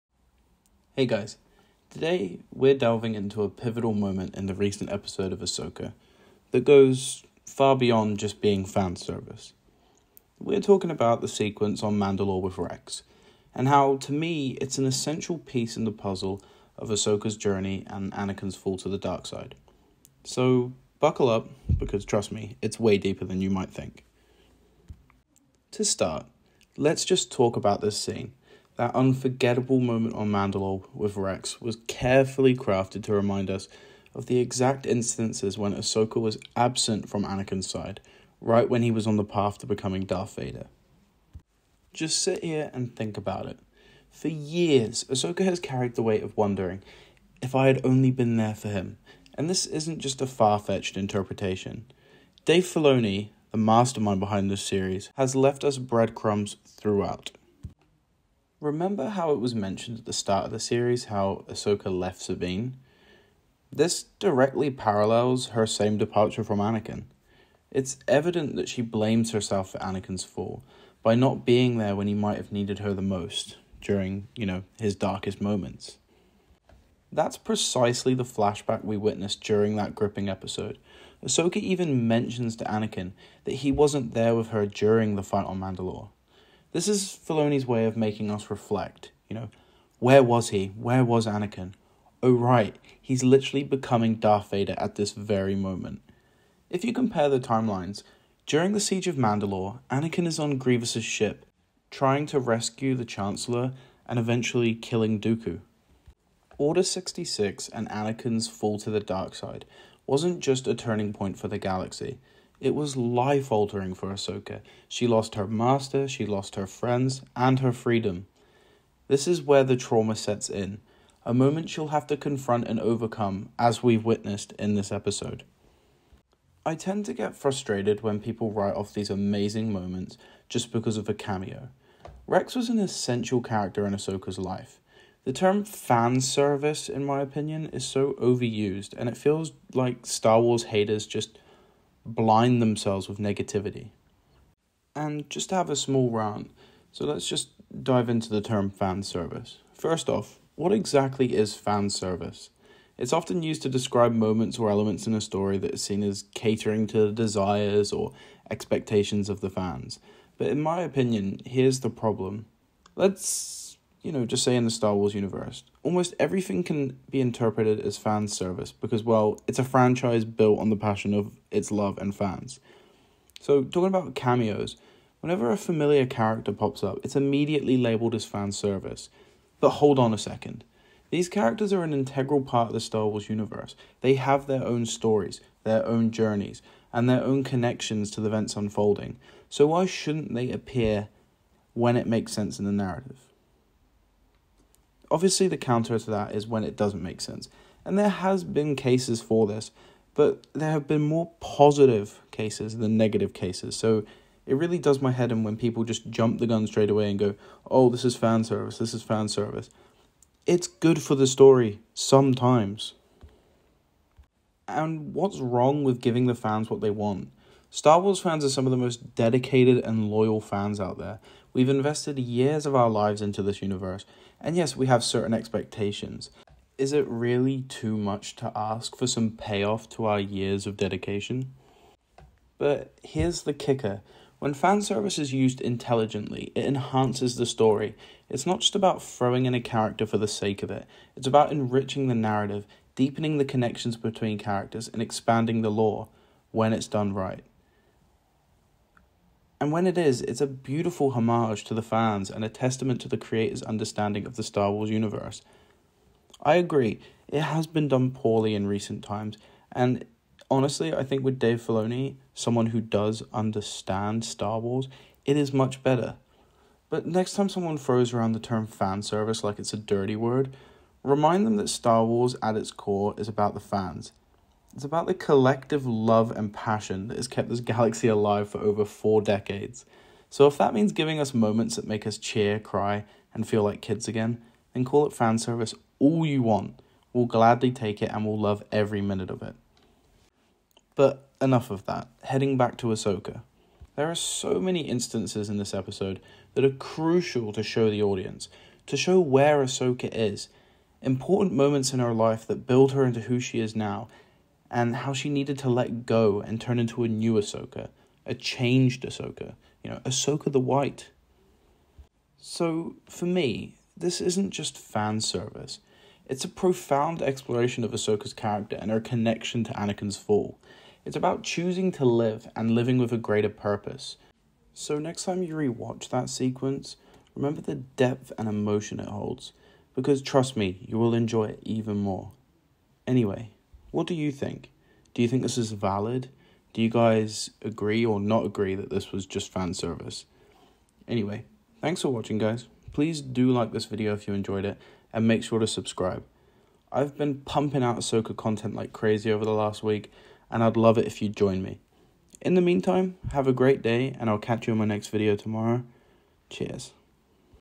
THIS! WASN'T JUST FAN SERVICE! | STAR WARS FANS and the term "FAN SERVICE"
In this video we break down so called "fan service" in AHSOKA, we talk about the uses of this "fan service" and if it really was or wasn't and the easy label fans like to put on it if they don't like what they are watching. We also breakdown the meaning for this for AHSOKA to grow and from this lesson learn to be AHSOKA THE WHITE!

Chapters:
0:00 - REX AND AHSOKA

/* VIDEO INFORMATION */

Hey Guys, Today, we're delving into a pivotal moment in the recent episode of Ahsoka that goes far beyond being just fan service. We're talking about the sequence on Mandalore with Rex, and how it's an essential piece in the puzzle of Ahsoka's journey and Anakin's fall to the dark side. So, buckle up, because it's way deeper than you might think!

To start, let's talk about this scene. That unforgettable moment on Mandalore with Rex was carefully crafted to remind us of the exact instances when Ahsoka was absent from Anakin's side, right when he was on the path to becoming Darth Vader.

Think about it. For years, Ahsoka has carried the weight of wondering, 'If only I had been there for him.' And this isn't just a far-fetched interpretation; Dave Filoni, the mastermind behind this series, has left us breadcrumbs throughout.

Remember how it was mentioned at the start of the series how Ahsoka left Sabine, directly paralleling her same departure from Anakin? It's evident that she blames herself for Anakin's fall by not being there when he might have needed her the most, during his darkest moment.

That's precisely the flashback we witnessed during that gripping episode. Ahsoka even mentions to Anakin that he wasn't there with her during the fight on Mandalore. This is Filoni's way of making us reflect, 'Where was he? Oh, right! He's becoming Vader at this very moment. If you compare the timelines, during the siege of mandalore Anakin is on Grievous's ship trying to rescue the Chancellor and eventually killing Dooku.

This moment wasn't just a turning point for the galaxy; it was life-altering for Ahsoka. She lost her master, her friends, her freedom. This is where the trauma sets in, a moment she'll have to confront and overcome, as we've witnessed in this episode.

BIG RANT HERE

I tend to get frustrated when people write off these amazing moments just because of a cameo. Rex was an essential character in Ashoka's life. The term “fan service” in my opinion is so overused and It feels like Star Wars haters just blind themselves with negativity. There are so many instances in this episode that are crucial to show the audience, to show where ahsoka was and is, important moments in her life that built her into who she is and how she needed this to let go and turn into a new Ahsoka, a changed Ahsoka, Ahsoka the white.

So for me, this isn't just fan service; it's a profound exploration of Ahsoka's character and her connection to Anakin's fall. It's about choosing to live and living with a greater purpose. So, next time you re watch that sequence, remember the depth and emotion it holds. Because trust me you will enjoy it even more. Anyway, what do you think? Do you think this is Valid? Do you or do you not agree that this was not just fan service? I've been pumping out Ahsoka content like crazy over the last week and id love it if you'd join me. In the meantime, have a great day and ill catch you in my next video tomorrow, cheers.

Hi If you made it down here!